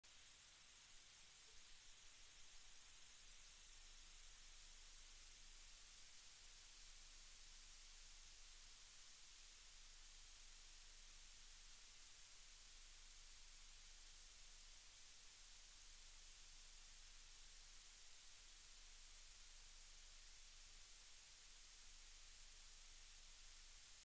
Newspaper Printing and Horatio Bottomley, 1910s - Film 1092085
World War One and anti-German sentiment 1910's. Shot of Horatio Bottomley. Printing press, newspapers coming off of conveyor belts. Bundles of newspapers wrapped in packing paper and tied with string move down a conveyor belt. The bundles of newspapers head towards the camera.